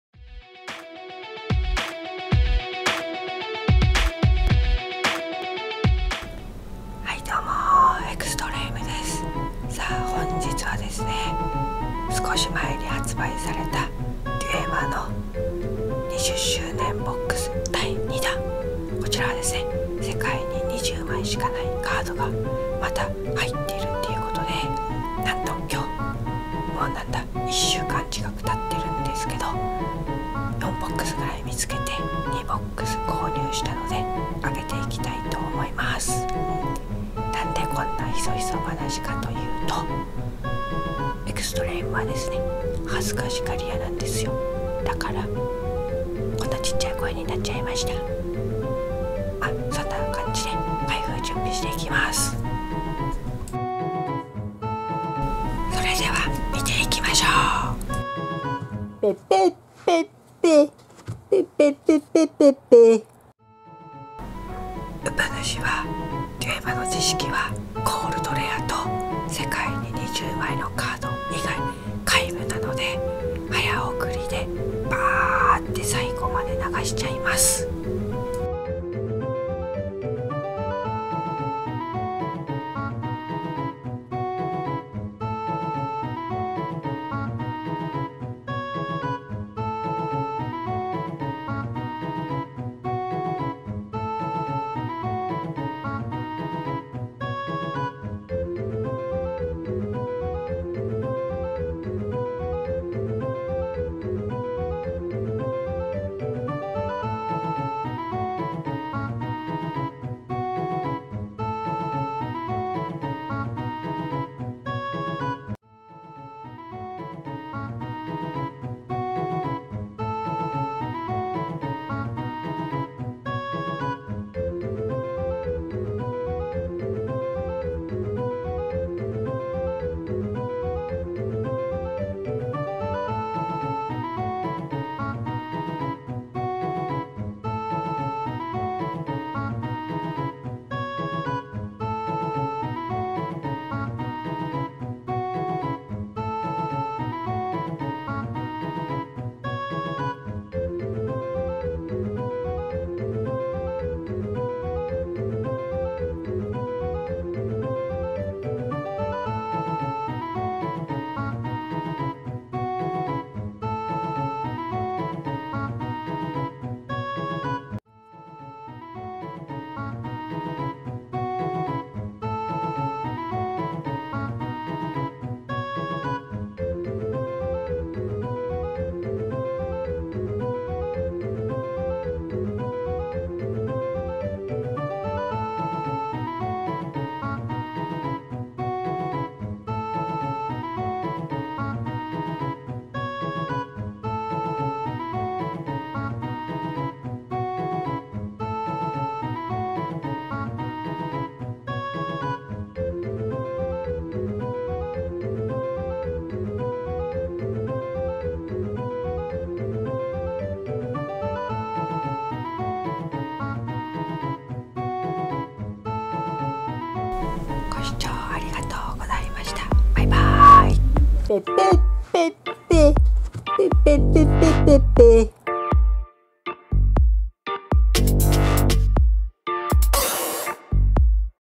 【デュエマ】シモカワさんに続け！20ｔｈ記念ボックス第二弾！開封していくっ！
いちをコレクターをやっているので

実は魚もおろせるんです！！

お仕事関係・お魚を提供してくださる方など
またはTwitter↓
DMまでよろしくお願いします。